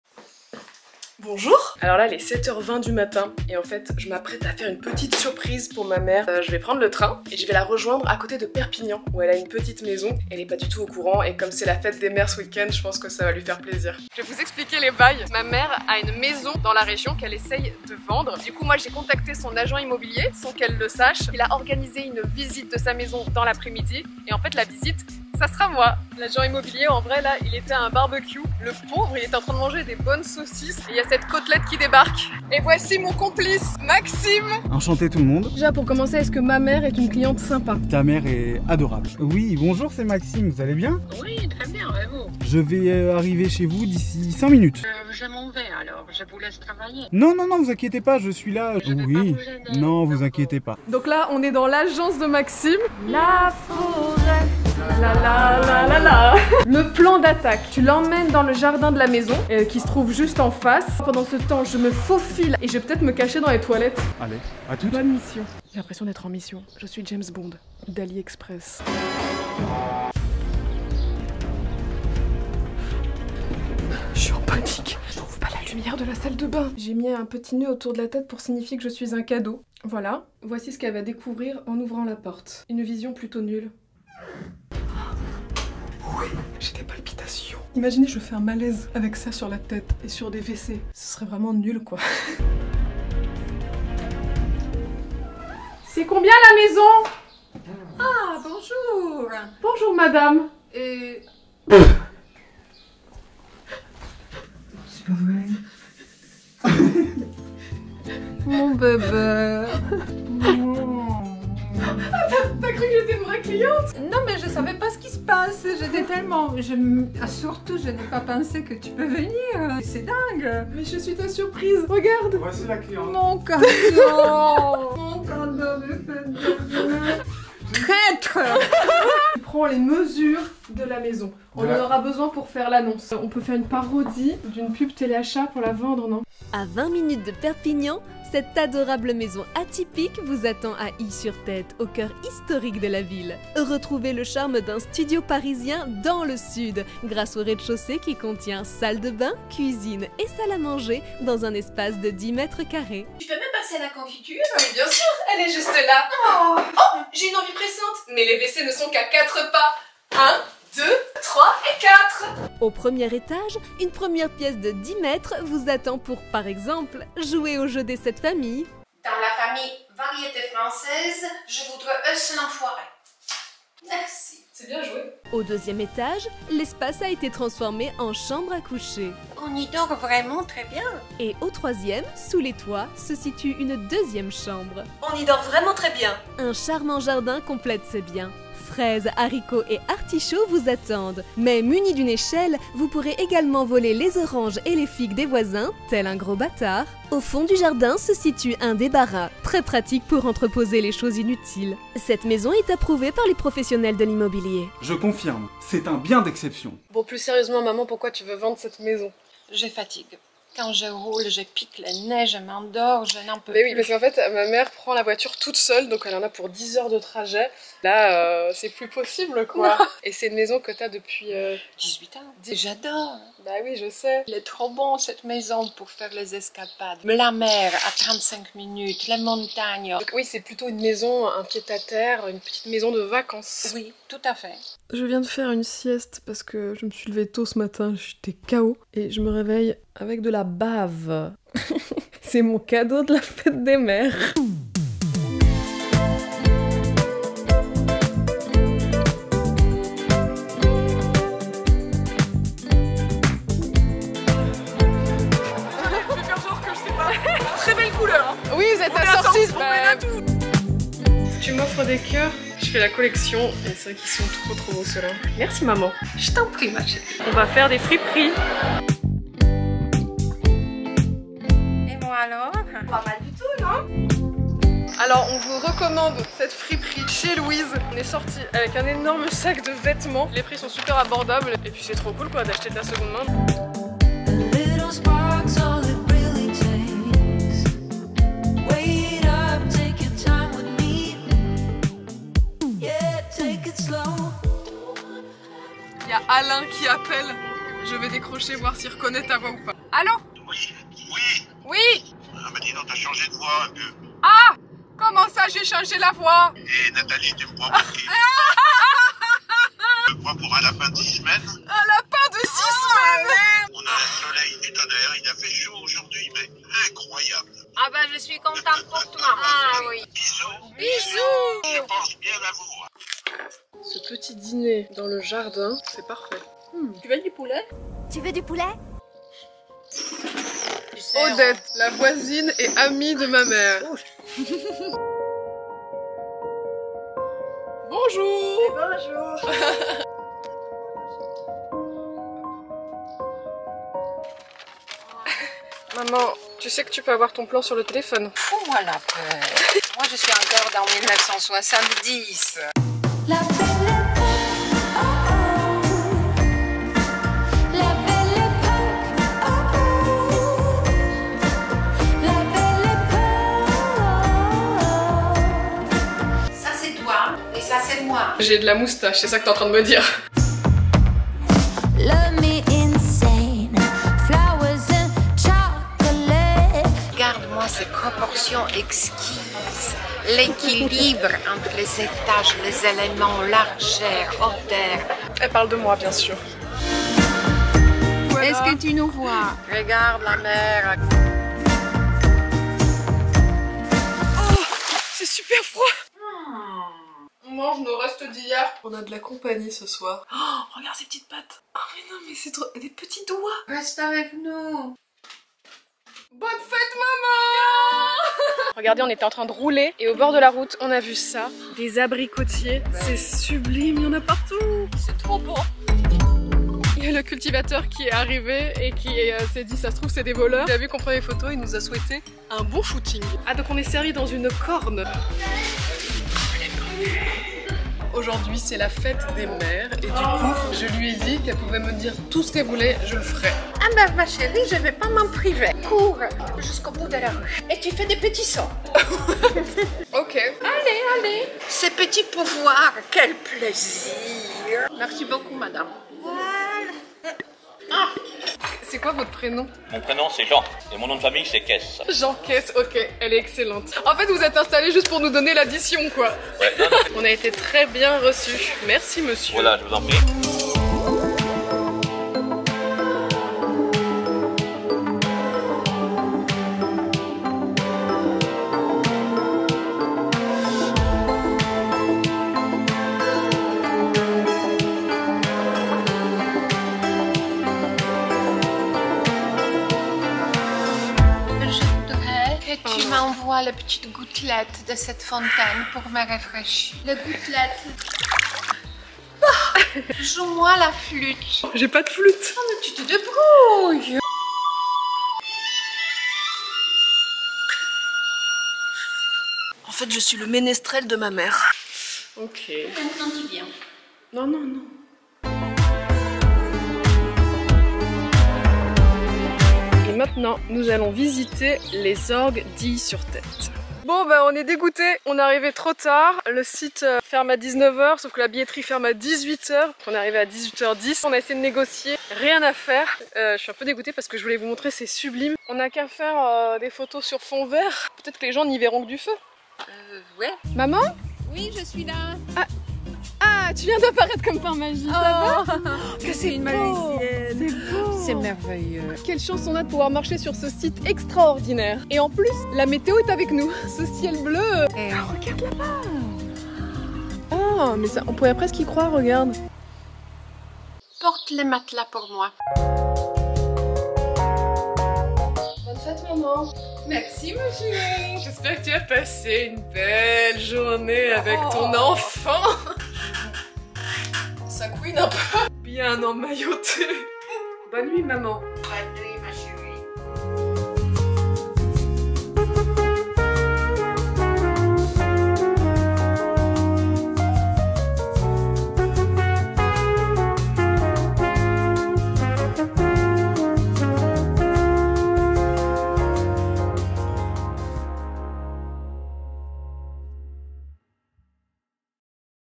Je surprends ma mère pour la fête des…mères 🎁
Je me suis totalement incrustée dans les vacances solo de ma mère : )